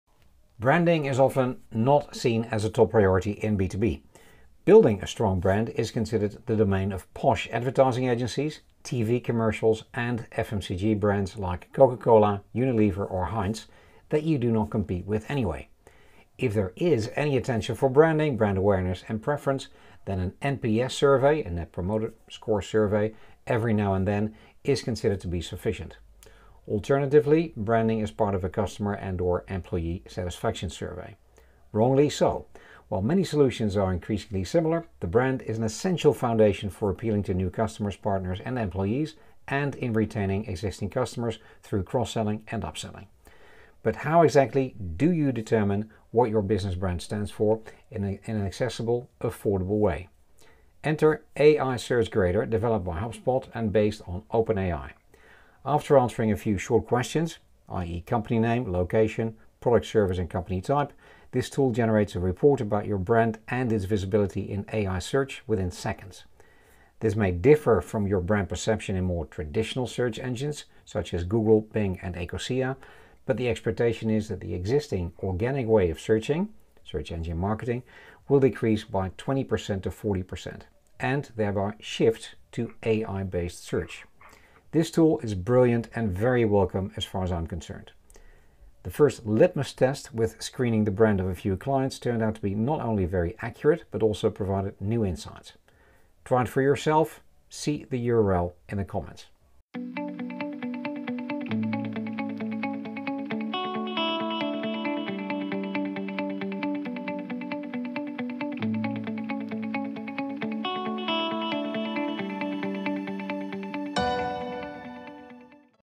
Check your B2B brand in minutes yourself? Enter AI Search Grader by HubSpot
In B2B, branding is often not a top priority.

Building a strong brand is considered the domain of posh advertising agencies, TV commercials and FMCG brands like Coca Cola, Unilever or Heinz that you do not compete with anyway.

If there is any attention for branding, brand awareness and preference, then an NPS survey every now and then is considered to be sufficient. Alternatively, branding is part of a customer and/or employee satisfaction survey.

Wrongly so. While many solutions are increasingly similar, the brand is an essential foundation for appealing to new customers, partners and employees and in retaining existing customers through cross-selling and up-selling.

But how do you determine what your business brand stands for in an accessible, affordable way?

Enter AI Search Grader, developed by HubSpot and based on Open AI. After answering a few short questions (company name, location, product/service and company type), this tool generates a report about your brand and its visibility in AI search within seconds.

This may differ from your brand perception in more 'traditional' search engines such as Google, BING and Ecosia, but the expectation is that the existing, organic way of searching (SEO) will decrease by 20% to 40%. And shift to AI-based search.

This tool is brilliant and very welcome as far as I'm concerned. The first litmus test with screening the brand of a few clients turned out to be not only very accurate, but also provided new insights. Try it yourself? See the URL